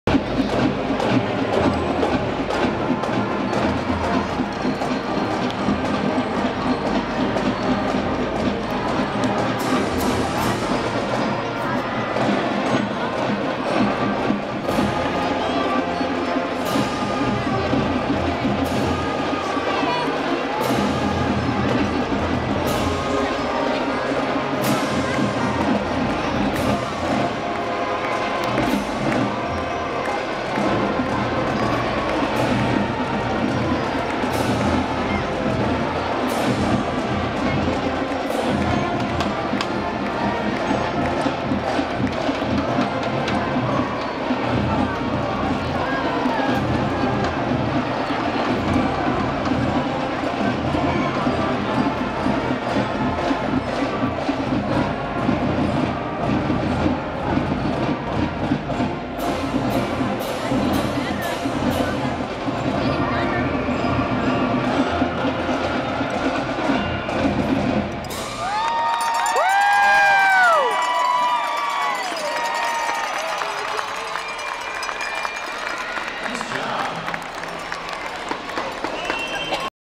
Actis Jr. High Band at Bakersfield Condors Game 2011
Members of the OJ Actis Junior High band played 3 songs at the Bakersfield Condors game in the Rabobank Arena, this is that final song in the 3rd period.